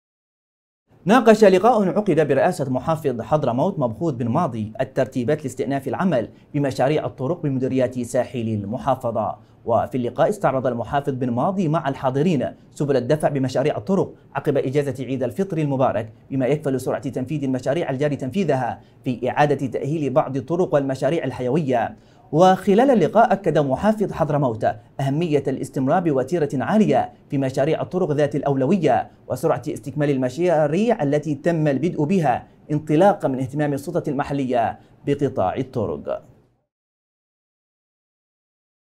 خبر | محافظ حضرموت يبحث الترتيبات لاستئناف العمل بمشاريع الطرق عقب إجازة عيد الفطر
ناقش لقاءٌ عُقد برئاسة محافظ حضرموت مبخوت بن ماضي الترتيبات لاستئناف العمل، بمشاريع الطُرق بمديريات ساحل المحافظة